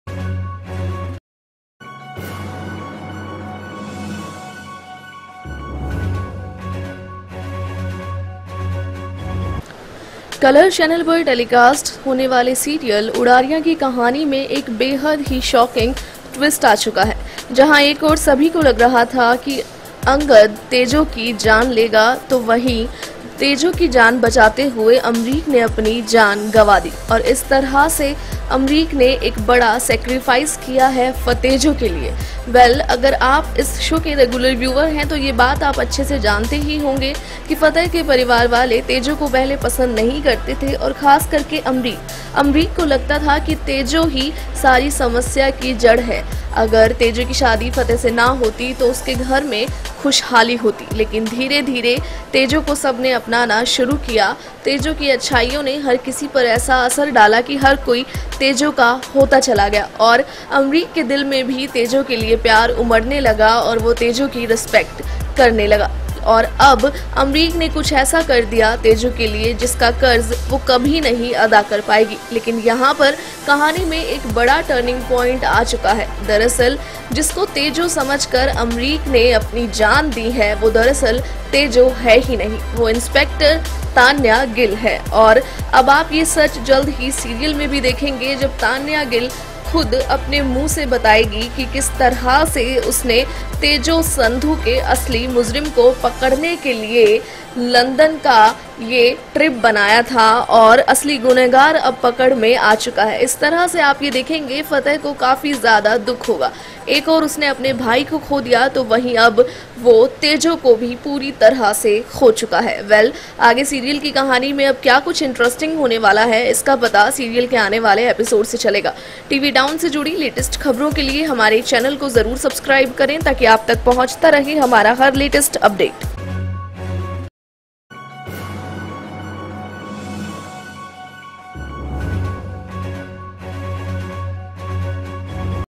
Udaariyaan Ep 374 Update: Amrik की मौत के बाद भी नहीं बदला कुछ, Tanya Gill उठाएगी सच से पर्दा U NEWS
Stat tuned for more tv Updates